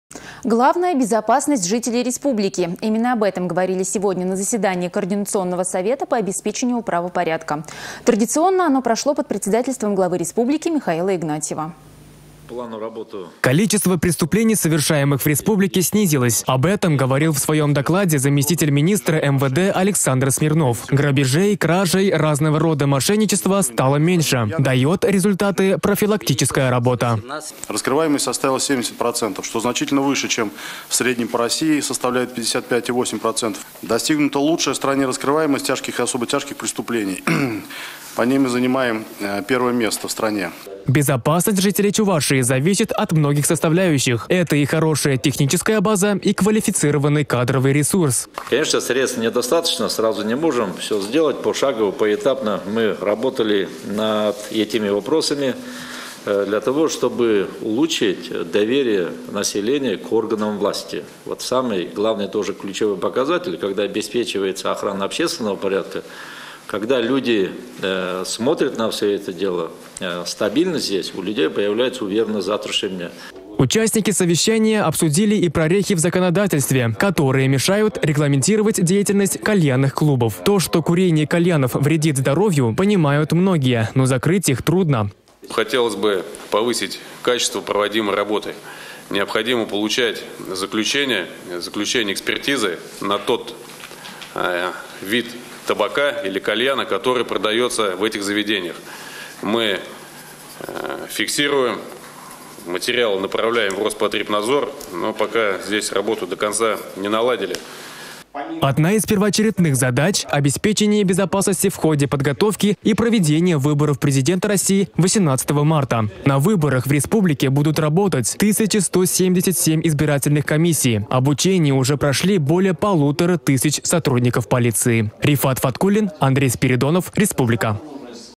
Главное – безопасность жителей республики.
Именно об этом говорили сегодня на заседании координационного совета по обеспечению правопорядка. Традиционно оно прошло под председательством главы республики Михаила Игнатьева.